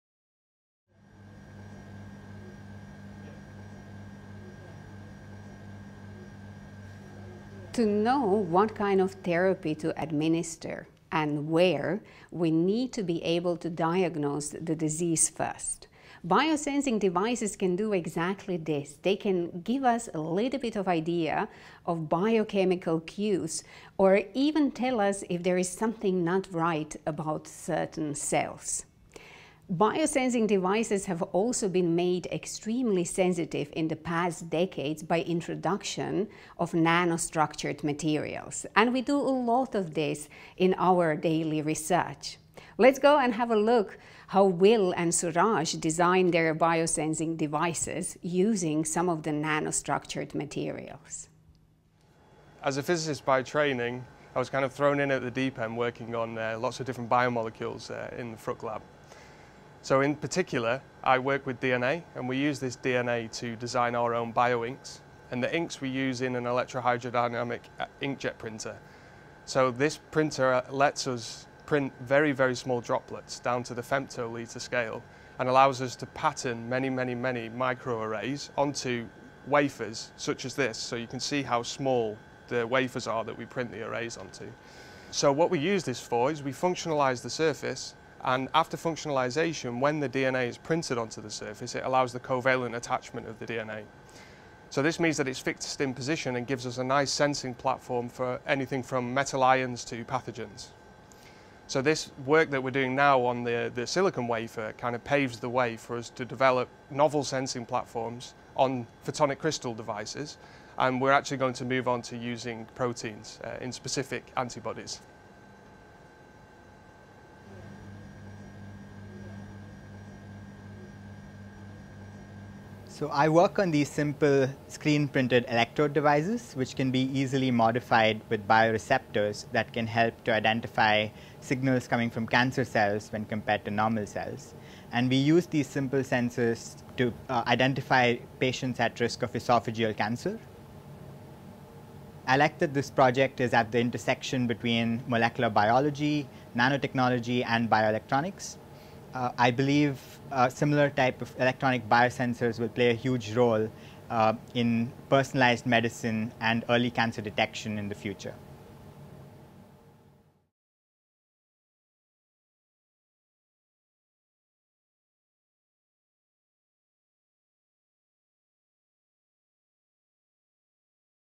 BioNano Engineering Group at CEB: Design of Biosensors
Biosensors can help us detect diseases earlier but also monitor our health and metabolism. New materials can help us design them to be more specific and easier to use.